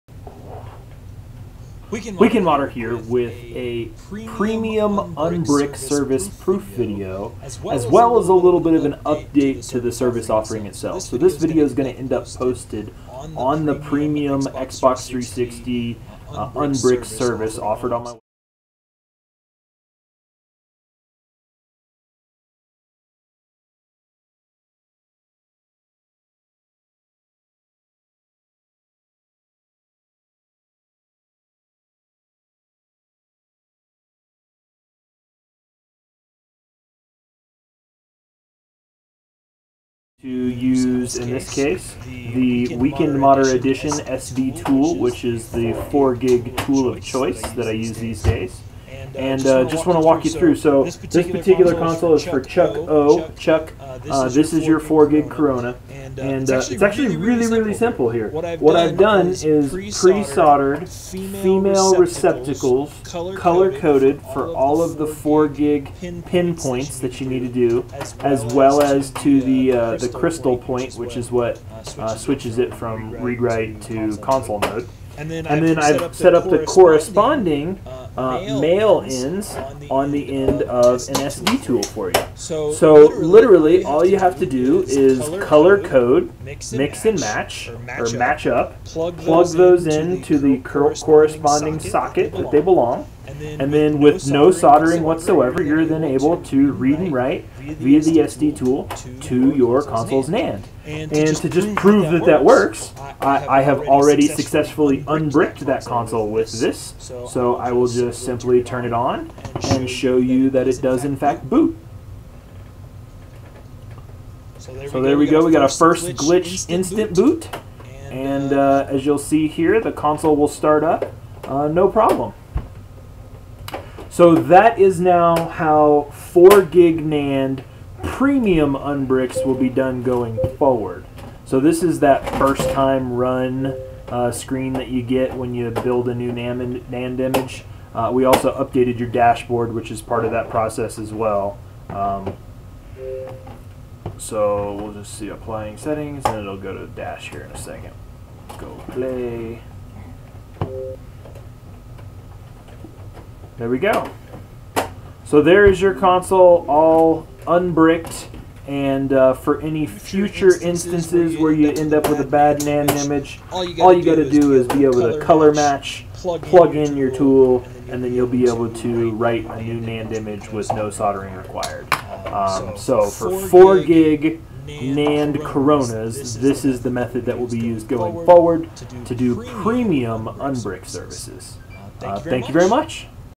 Corona 4gb Premium Unbrick Service - unbrick yourself with no soldering!
The dupont connectors I used to make this can be purchased here if you want to make your own